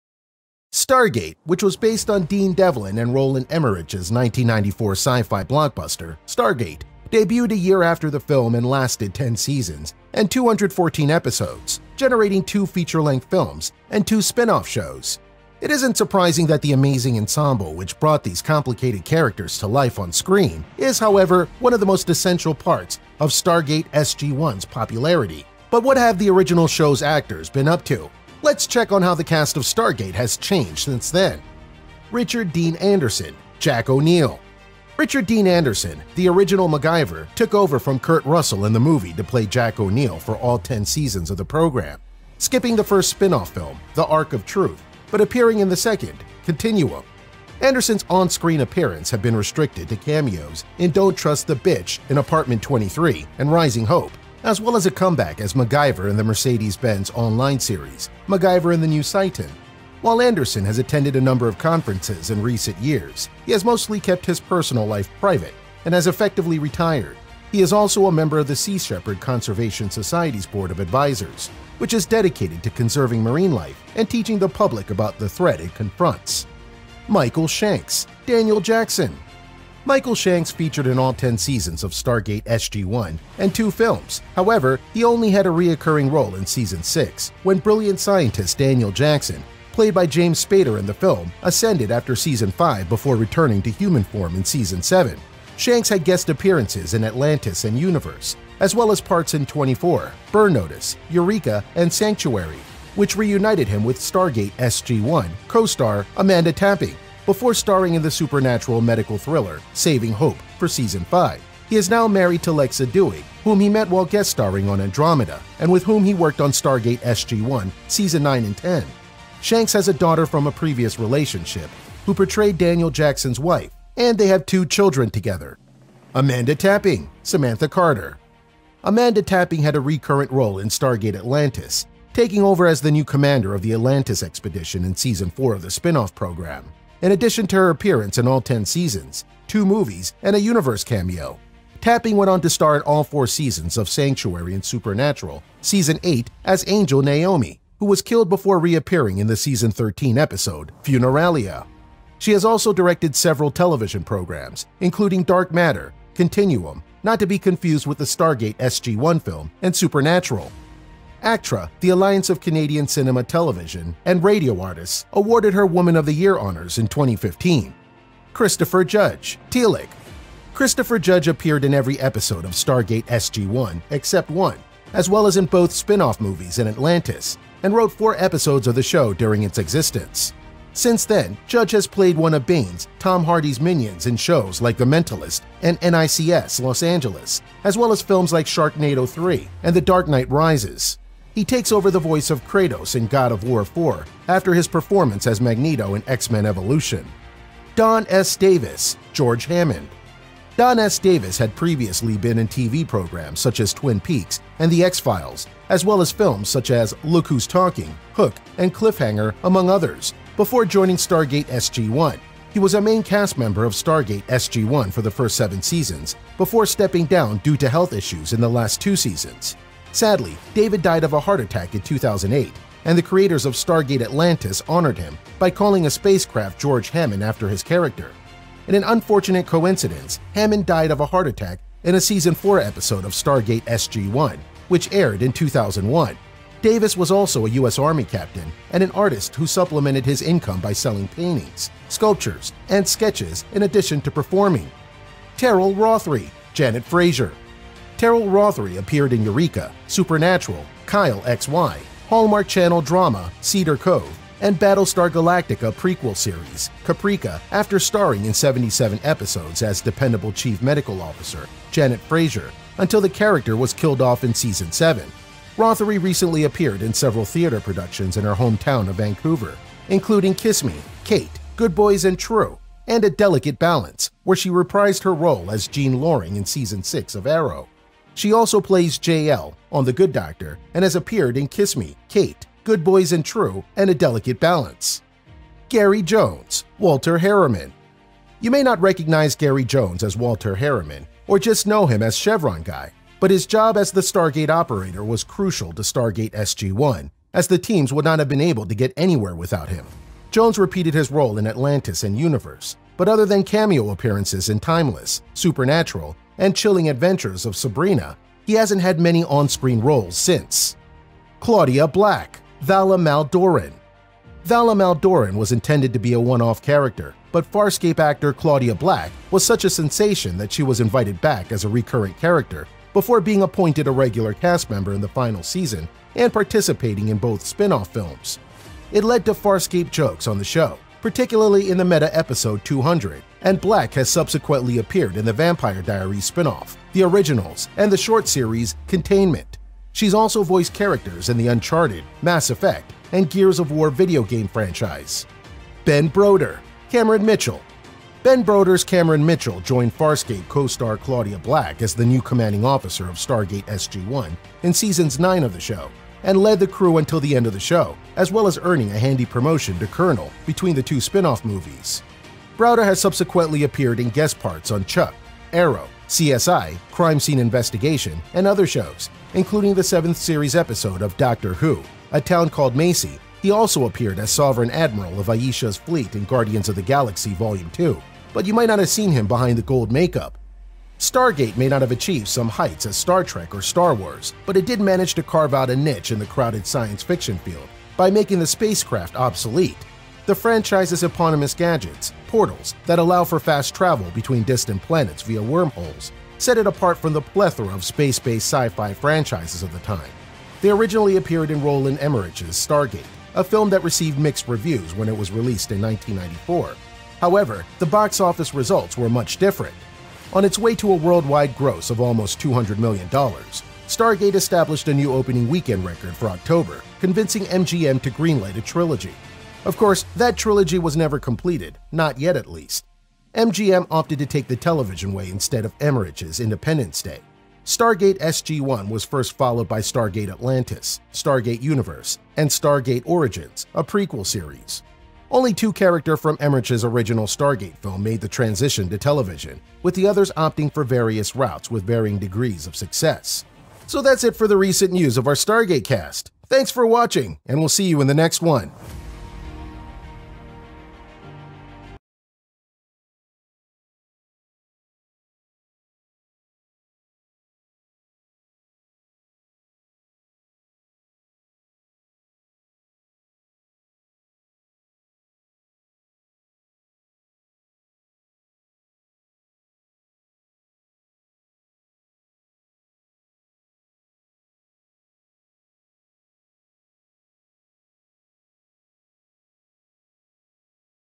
The Cast of Stargate Then and Now
Stargate, which was based on Dean Devlin and Roland Emmerich's 1994 sci-fi blockbuster Stargate, debuted a year after the film and lasted ten seasons and 214 episodes, generating two feature-length films and two spin-off shows. It isn’t surprising that the amazing ensemble which brought these complicated characters to life on screen is, however, one of the most essential parts of Stargate SG-1's popularity. But what have the original show's actors been up to? Let’s check on how the cast of Stargate has changed since then.

Richard Dean Anderson – Jack O'Neill

Richard Dean Anderson, the original MacGyver, took over from Kurt Russell in the movie to play Jack O'Neill for all ten seasons of the program, skipping the first spin-off film The Ark of Truth but appearing in the second, Continuum.
Anderson's on-screen appearances have been restricted to cameos in Don't Trust the B—— in Apartment 23 and Raising Hope, as well as a comeback as MacGyver in the Mercedes-Benz online series MacGyver and the New Citan.

While Anderson has attended a number of conferences in recent years, he has mostly kept his personal life private and has effectively retired. He is also a member of the Sea Shepherd Conservation Society's Board of Advisors, which is dedicated to conserving marine life and teaching the public about the threats it confronts.

Michael Shanks – Daniel Jackson

Michael Shanks featured in all ten seasons of Stargate SG-1 and two films, however, he only had a recurring role in season six when brilliant scientist Daniel Jackson (played by James Spader in the film) ascended after season five before returning to human form in season seven.

Shanks had guest appearances in Atlantis and Universe, as well as parts in 24, Burn Notice, Eureka, and Sanctuary, which reunited him with Stargate SG-1 co-star Amanda Tapping, before starring in the supernatural medical thriller Saving Hope for five seasons.

He is now married to Lexa Doig, whom he met while guest-starring on Andromeda, and with whom he worked on Stargate SG-1 seasons nine and ten. Shanks has a daughter from a previous relationship with Vaitiare Bandera, who portrayed Daniel Jackson's wife Sha're, and they have two children together.

Amanda Tapping – Samantha Carter

Amanda Tapping had a recurrent role in Stargate Atlantis, taking over as the new commander of the Atlantis expedition in season four of the spin-off program, in addition to her appearances in all ten seasons, two movies, and a Universe cameo.

Tapping went on to star in all four seasons of Sanctuary and Supernatural season eight as angel Naomi, who was killed off before reappearing in the season 13 episode 'Funeralia.' She has also directed several television programs, including Dark Matter, Continuum (not to be confused with the Stargate SG-1 film), and Supernatural.

ACTRA, the Alliance of Canadian Cinema, Television, and Radio Artists, awarded her Woman of the Year honors in 2015.

Christopher Judge – Teal'c

Christopher Judge appeared in every episode of Stargate SG-1 except one, as well as in both spin-off movies and Atlantis, and wrote four episodes of the show during its existence.

Since then, Judge has played one of Bane's (Tom Hardy) minions in shows like The Mentalist and NCIS: Los Angeles, as well as films like Sharknado 3 and The Dark Knight Rises. He takes over as the voice of Kratos in God of War 4 after his performance as Magneto in X-Men: Evolution.

Don S Davis – George Hammond

Don S Davis had previously been in TV programs such as Twin Peaks and The X-Files, as well as films such as Look Who's Talking, Hook, and Cliffhanger, among others, before joining Stargate SG-1. He was a main cast member of Stargate SG-1 for the first seven seasons before stepping down due to health issues in the last two seasons.

Sadly, Davis died of a heart attack in 2008, and the creators of Stargate Atlantis honored him by calling a spacecraft George Hammond after his character. In an unfortunate coincidence, Hammond died of a heart attack in a season four episode of Stargate SG-1 which aired in 2001.

Davis was also a U.S. Army captain and an artist who supplemented his income by selling paintings, sculptures, and sketches in addition to performing.

So that’s it for the recent news of our Stargate cast. I'll see you on the channel next time!